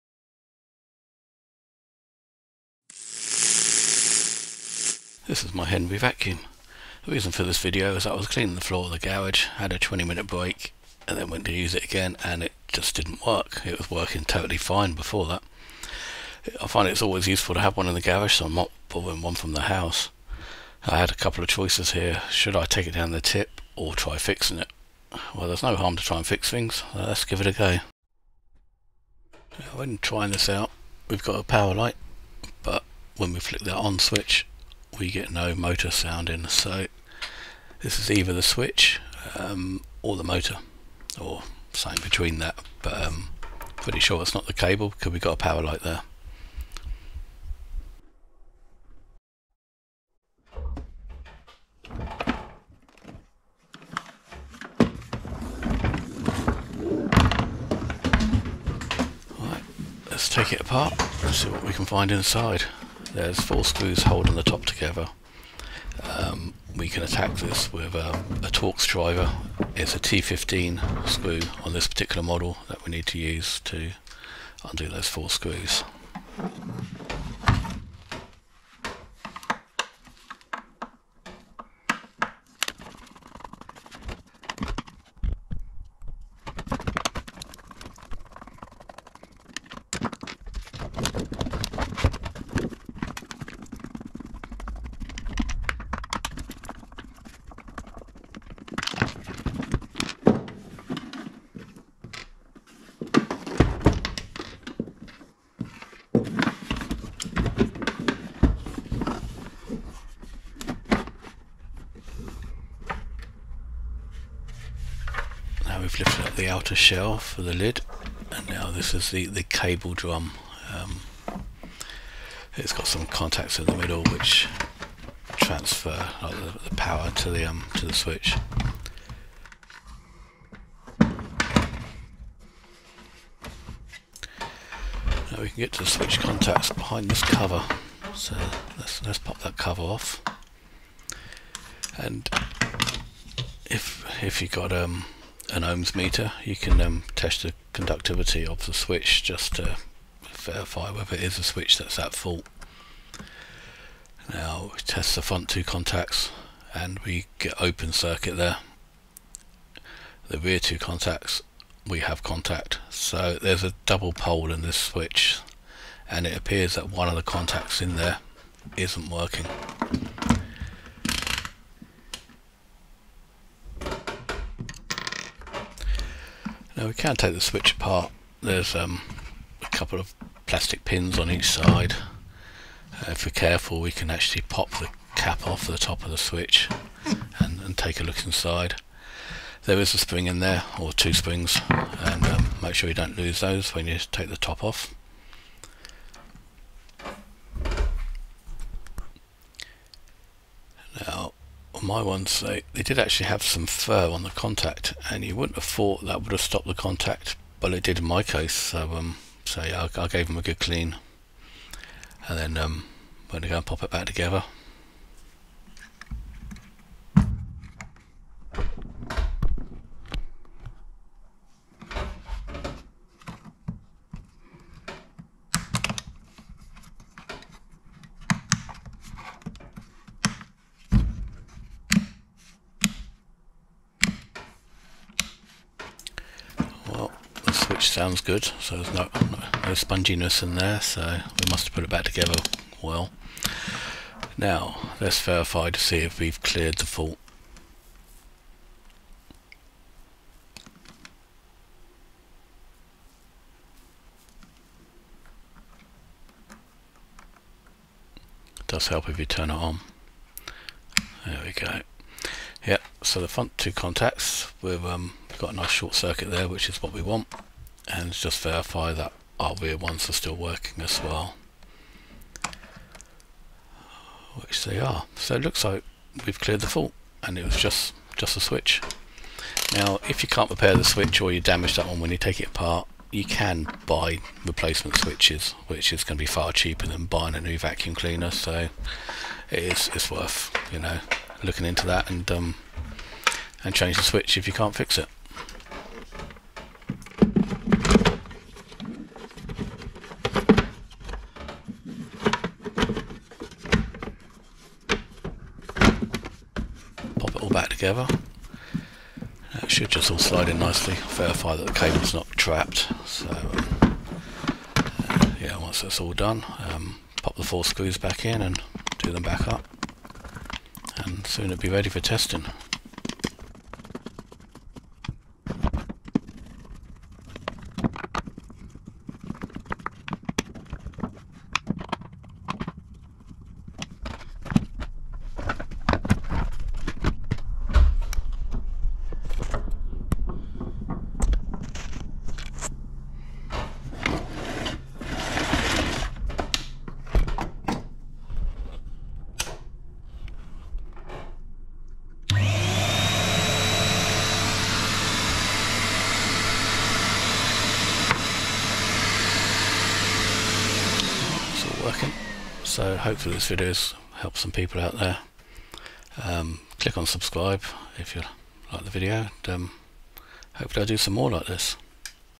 Henry Hoover Vacuum Switch Fix
My Henry died sudenly. No symptoma before. Was going to chuck it and get another. They are great units. Then though, well how about trying to repair it. Power light was on, but the switch didn't start the motor. Was it a broken wire, crimp come off, brushes in the motor, or a bad switch?.
In my case it was just a dirty contact in the switch. Cleaned it out and all good again now.